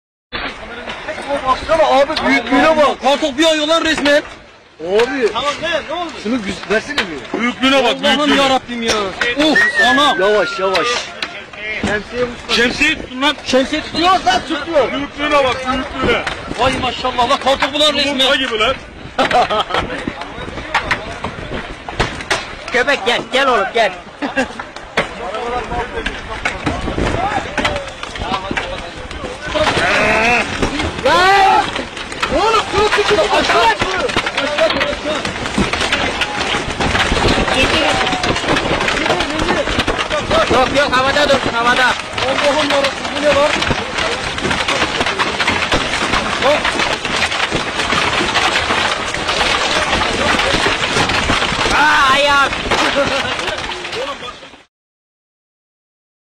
Eskihisar'da büyük kütleli dolu yağışı şaşırttı
Kocaeli'nin Gebze İlçesi'nde Eskihisar sahilinde çekilerek sosyal medyada paylaşılan görüntüler ilgi çekti.

Sahilde ve denizde etkili olan dolu yağışında denize düşen doluların suyu havalandırması ve doluların büyüklüğü dikkat çekti. Doluların denize düşmesiyle oluşan görüntüler şaşkınlıkla izlenildi. Çekim yapanlar saçakların altına girdi.